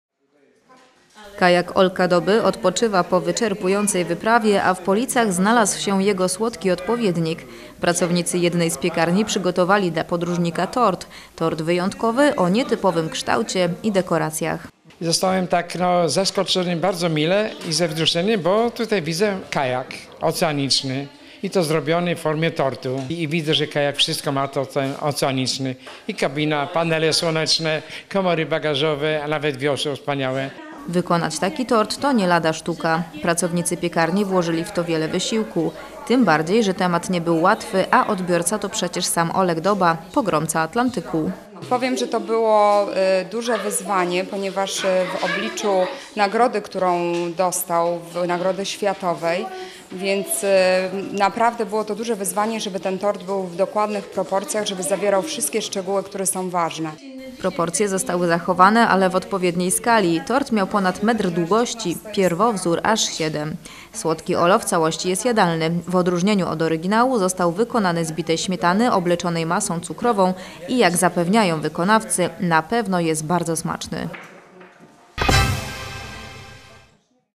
Niezwykły tort dla Olka Doby.
Infopolice 03.03.03r. - Jedna z polickich piekarni przygotowała słodki poczęstunek dla Olka Doby. Miły gest nie byłby tak niezwykły gdyby  tort nie wyglądał jak prawdziwa rzeźba!